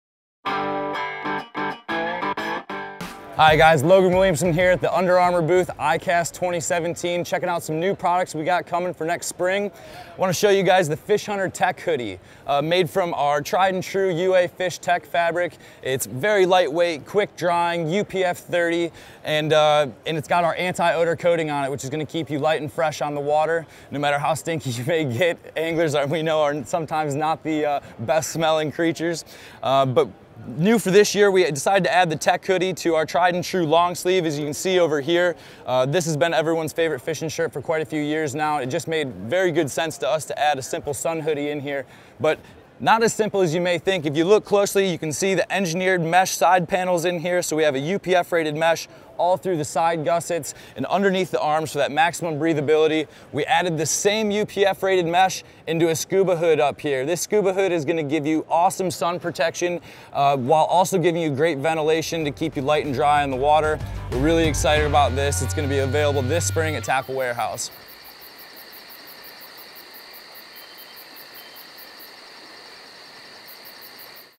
Under Armour Fish Hunter Tech Hoodie | ICAST 2017
Tackle Warehouse discusses the new Under Armour Fish Hunter Tech Hoodie at the 2017 ICAST Show in Orlando.